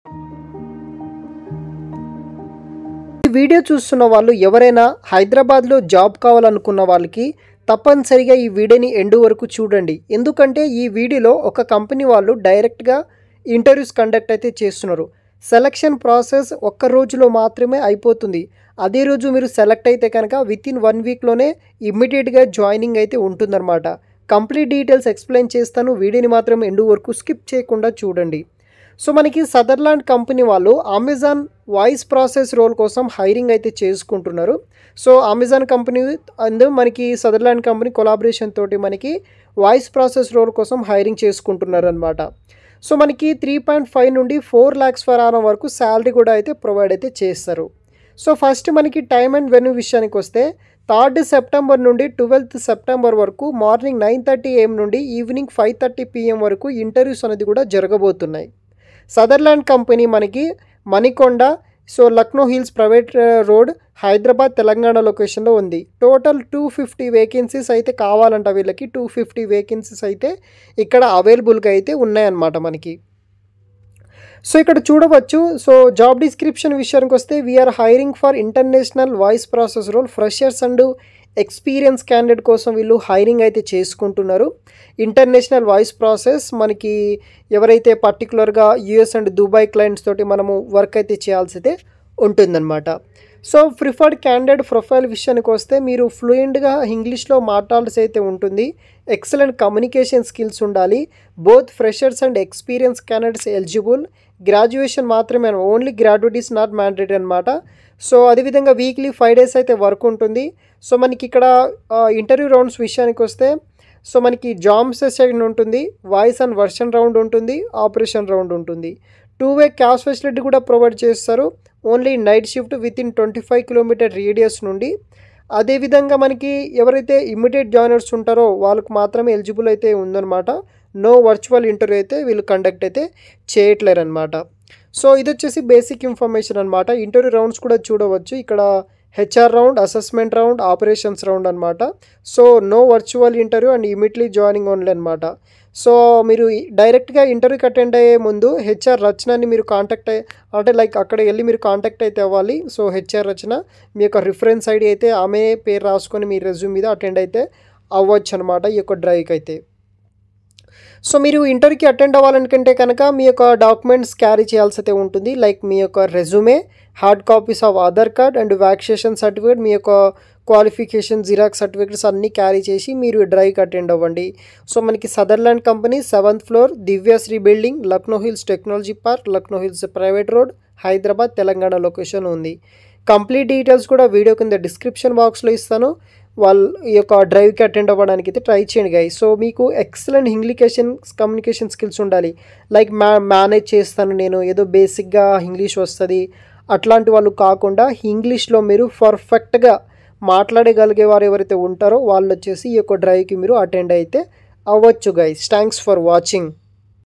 Sutherland Company Walk In Interviews In Hyderabad | Jobs In Hyderabad | Latest Private jobs
Looking for job opportunities in Hyderabad? Watch our latest video on Sutherland and Amazon walk-in interviews for 2024. Learn about the job details and requirements to help you ace the interview process.

✅Apply Link Here:-
👉 3rd Sep - 12th Sep- 2024
👉Sutherland, 7th floor, Divya Sree building, Lanco Hills Technology Park Lanco Hills Private Rd, Hyderabad, Telangana.
Note: Direct Walk In Interviews